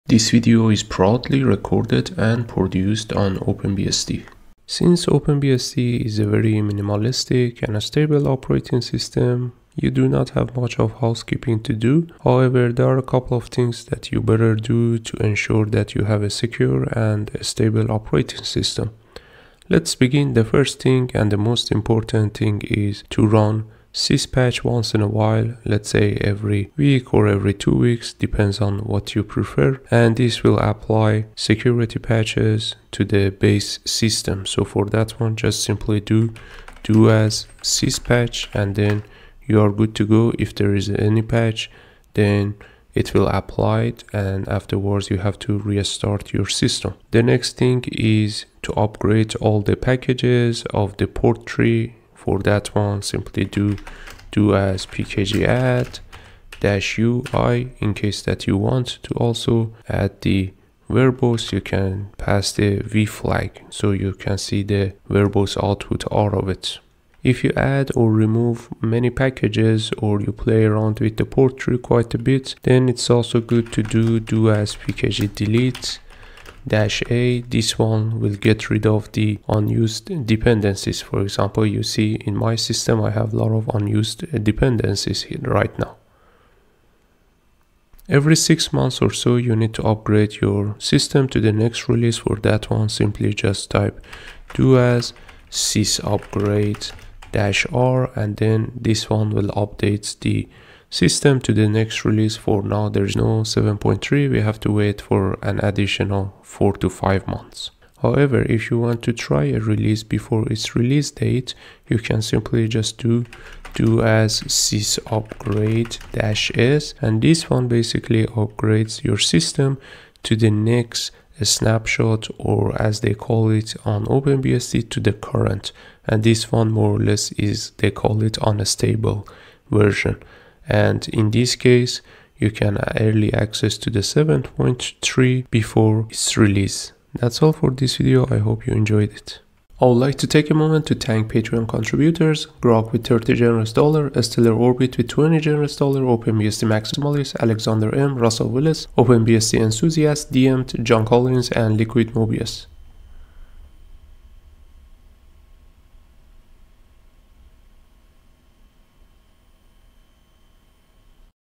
OpenBSD housekeeping
There is not much housekeeping to do on OpenBSD but it is good to run a couple of commands every once in a while, like a week, to keep your system secure and clean.